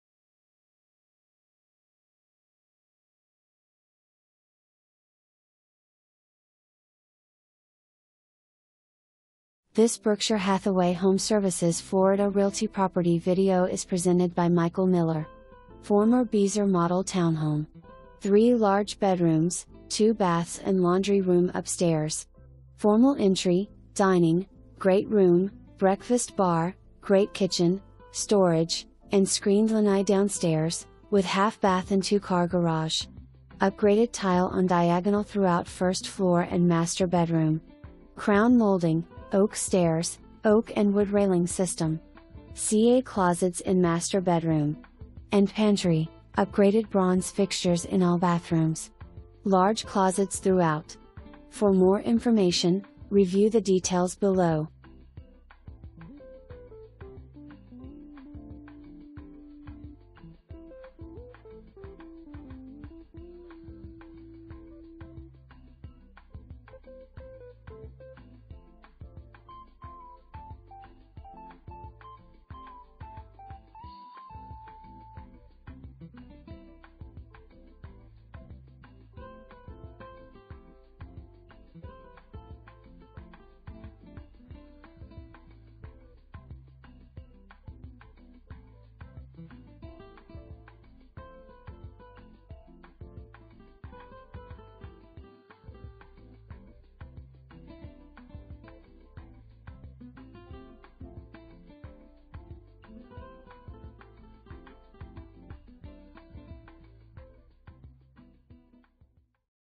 Berkshire Hathaway HomeServices Florida Realty - 19571 Bowring Park RD #103
19571 Bowring Park RD #103

Former Beazer Model townhome. 3 large bedrooms, two baths and laundry room upstairs. Formal entry, Dining, great room, breakfast bar, great kitchen, storage, and screened lanai downstairs, with 1/2 bath and two car garage.. Upgraded tile on diagonal throughout first floor and Master bedroom. Crown molding, Oak stairs, Oak and wood railing system. CA closets in master bedroom. and pantry, upgraded bronze fixtures in all bathrooms. Large closets throughout.

Michael Miller
Berkshire Hathaway HomeServices Florida Realty